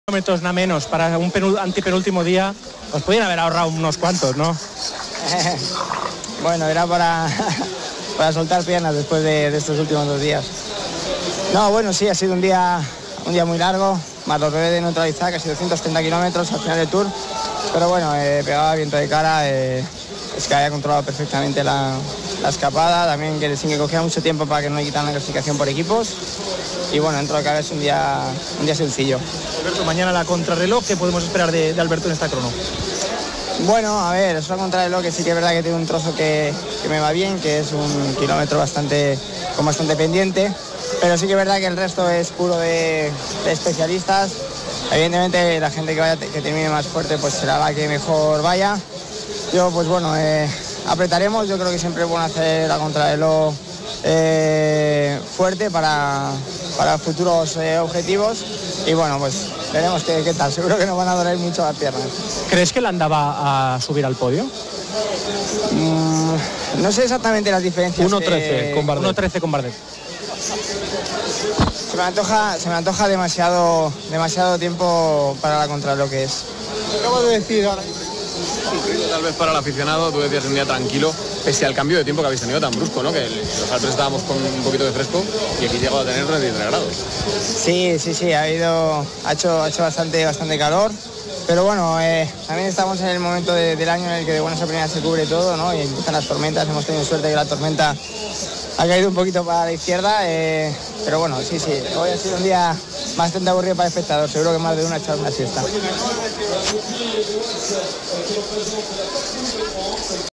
Alberto Contador | MAÑANA ME VAN A DOLER MUCHO LAS PIERNAS | Etapa 19 | TOUR DE FRANCIA 2017
Declaraciones de Alberto Contador una vez finalizada la Etapa 19 de este Tour de Francia 2017.